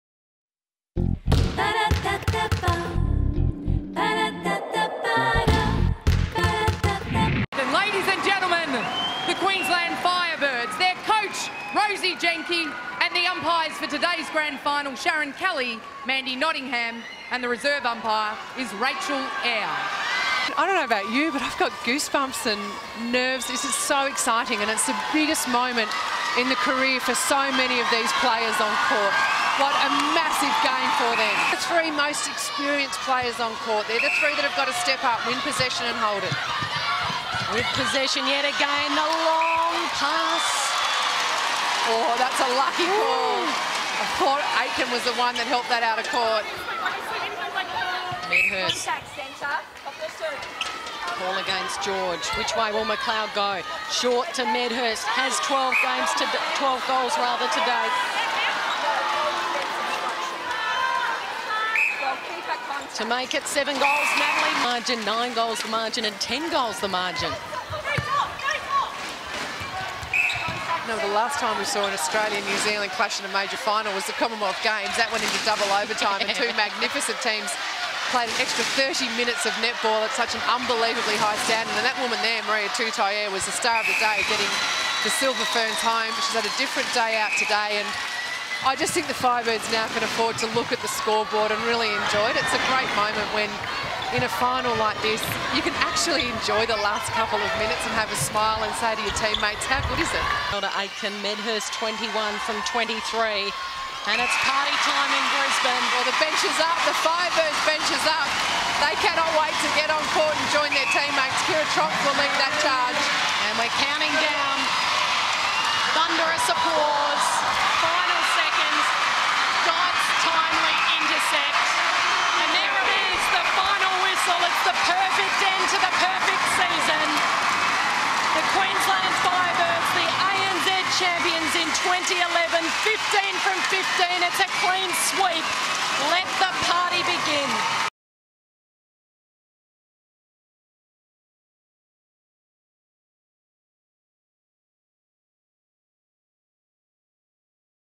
Firebirds 2011 Premiers
Get ready for the 2012 ANZ Championship season by reliving highlights from the 2011 Grand Final.